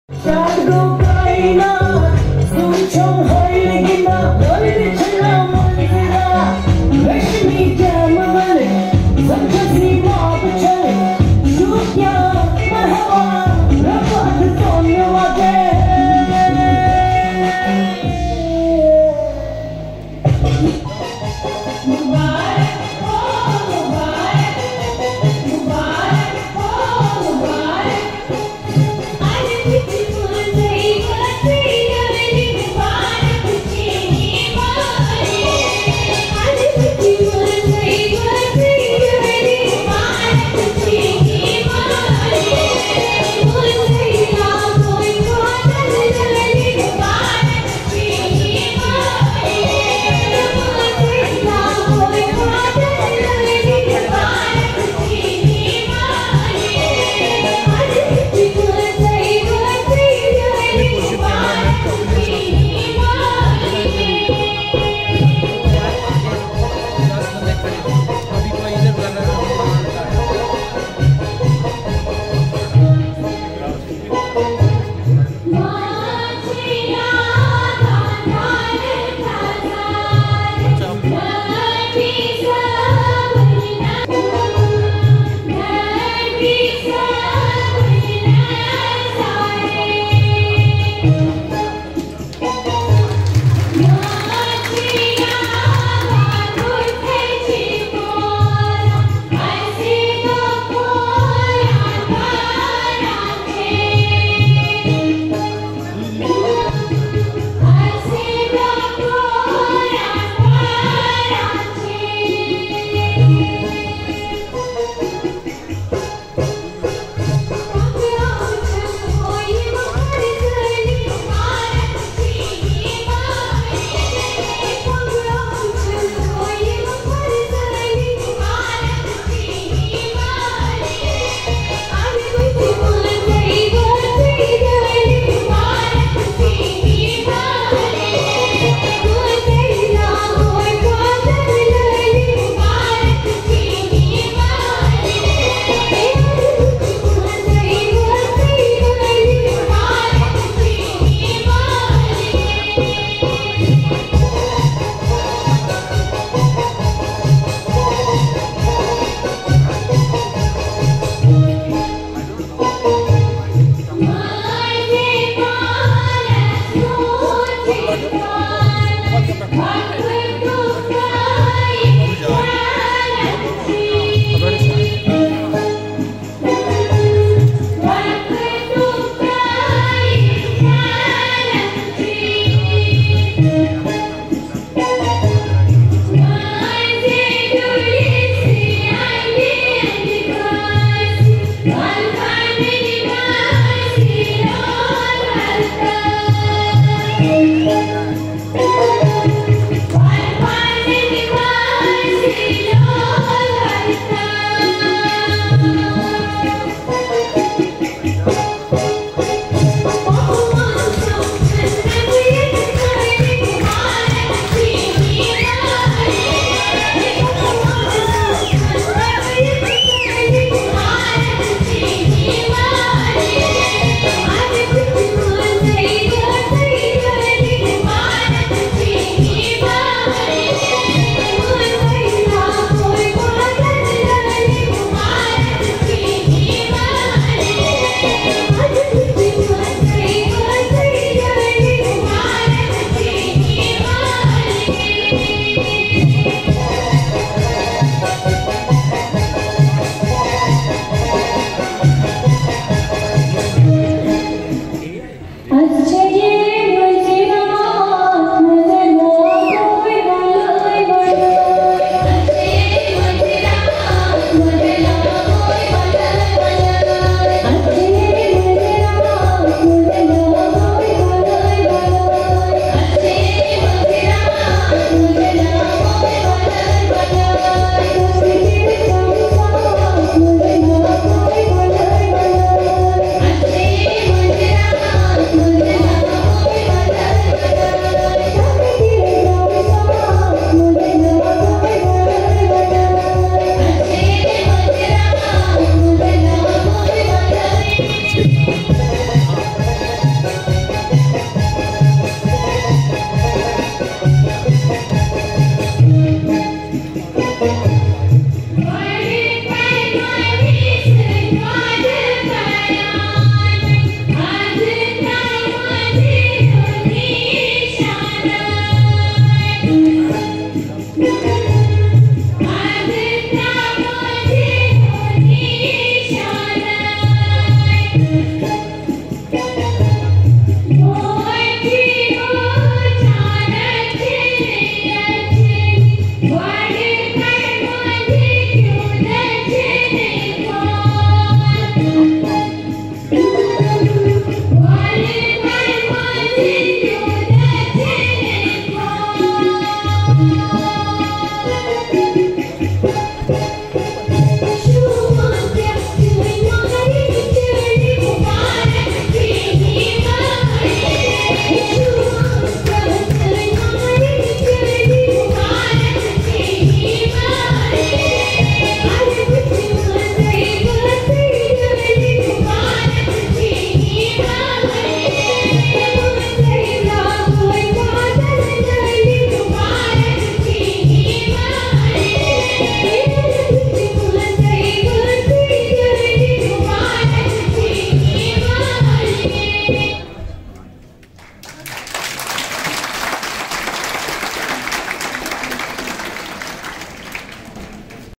19th Foundation Day of Baba Ghulam Shah Badshah University Rajouri
celebration of 19th Foundation day of Baba Ghulam Shah Badshah University Rajouri,
kashmiri Roff